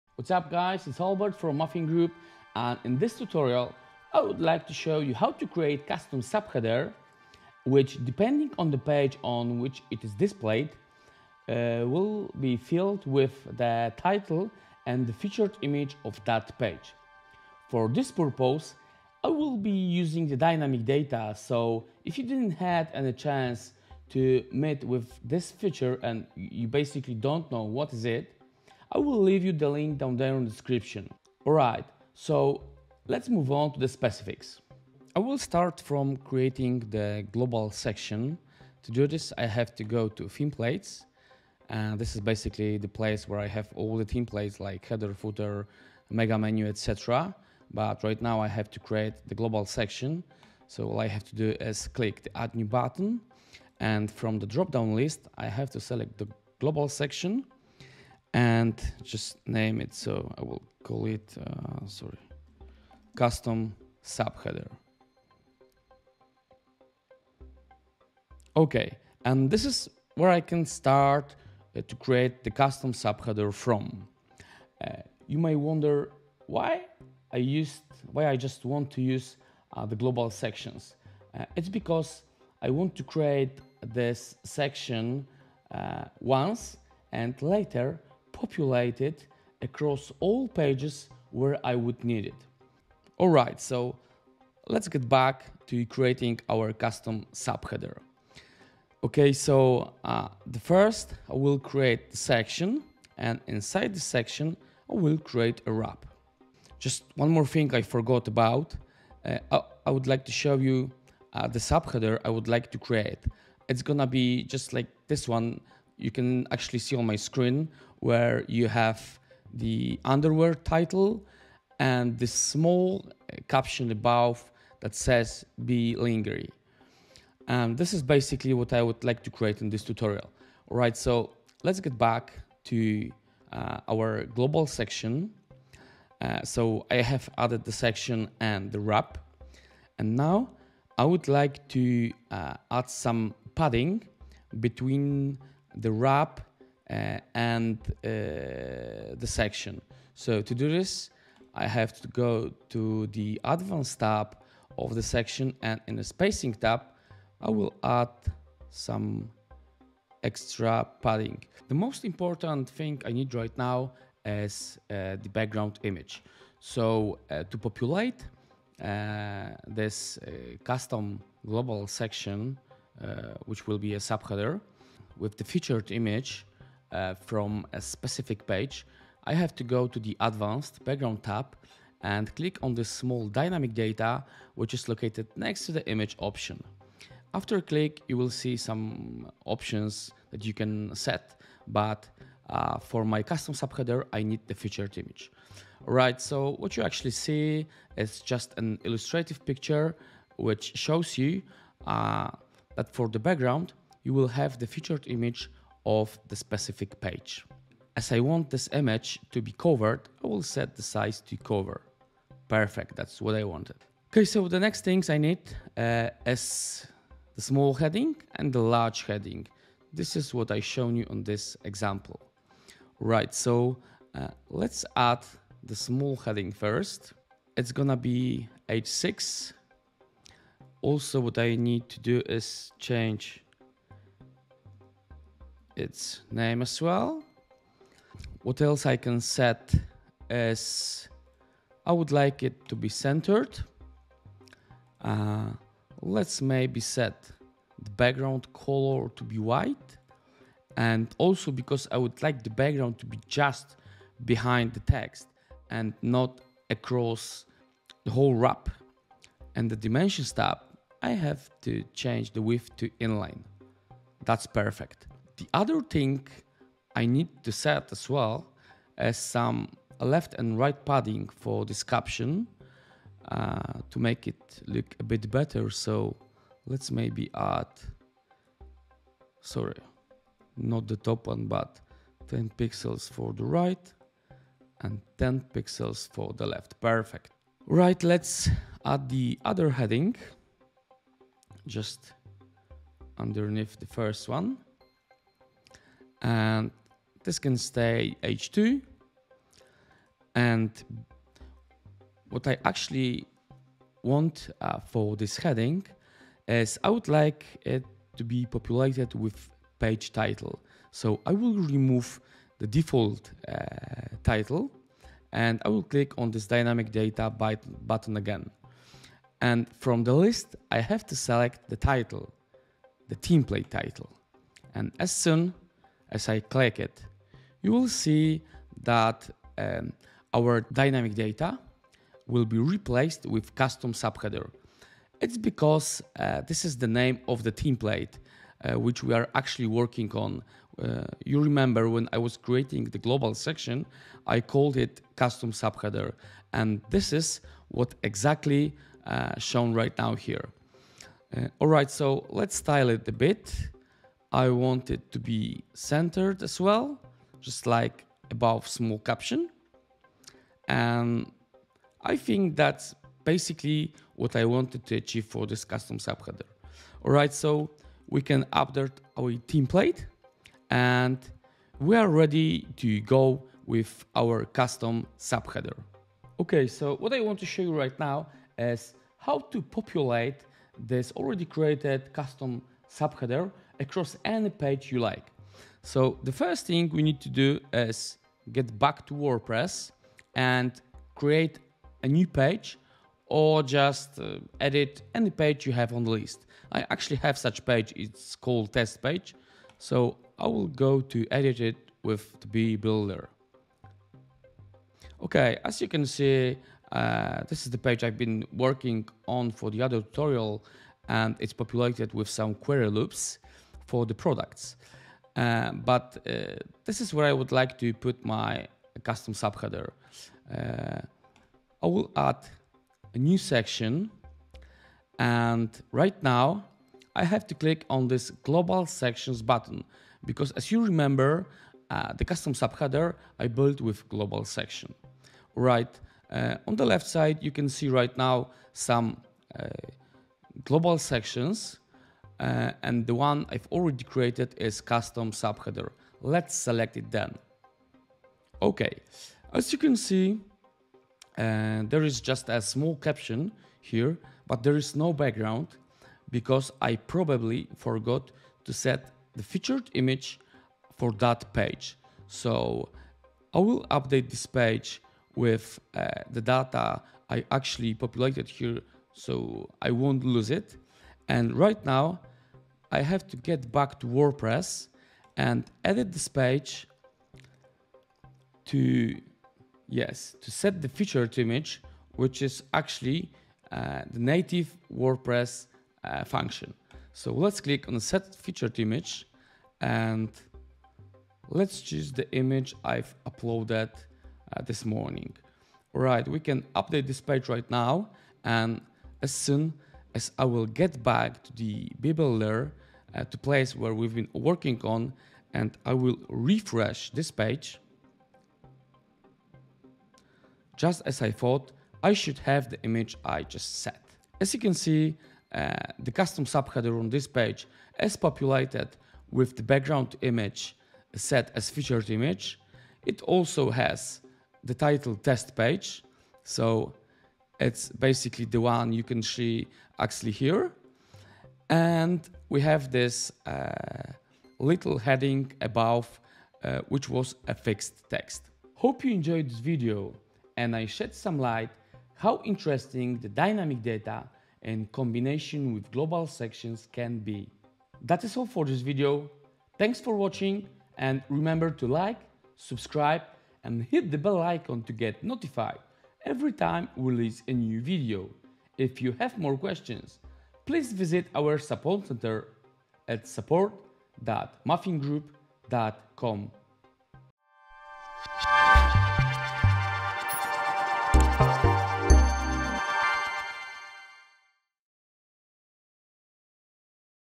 Custom Subheader with Dynamic Data
In this tutorial i would like to show you how to create custom subheader which, depending on the page on which it is displayed, is filled with the title and featured image of that page. For this purpose, I will use Global Sections and Dynamic Data. If you didn't had a chance to meet these two, please check the video tutorials below: